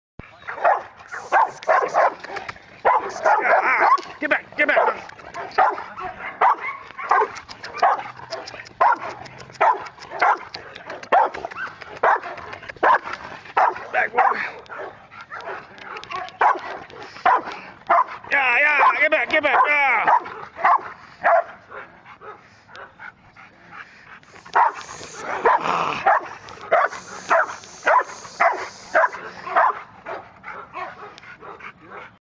Rottweiler Sasha training in Area Protection..
K9 Enforcement's Sasha ,an 8 month old Female Rottweiler.
Sasha is being trained here on her area protection.